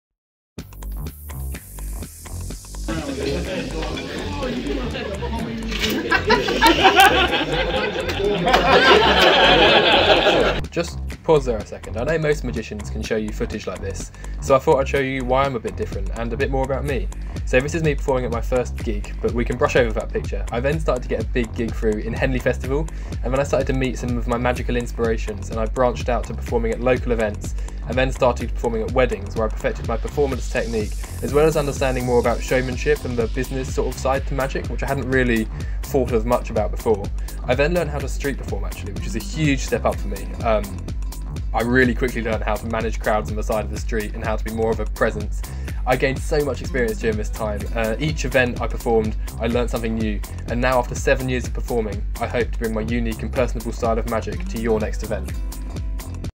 Sam The Magician - Magician For Hire Hertfordshire - AliveNetwork.com
Adding an air of excitement to your special occasion, Sam The Magician is a passionate entertainer who honed his craft from a very young age! Performing incredible sleight of hand & close up magic, Sam will wow all your guests whether they are 5 or 95! A real hit with Weddings, Private Parties, Restaurants & Corporate Event's across the UK!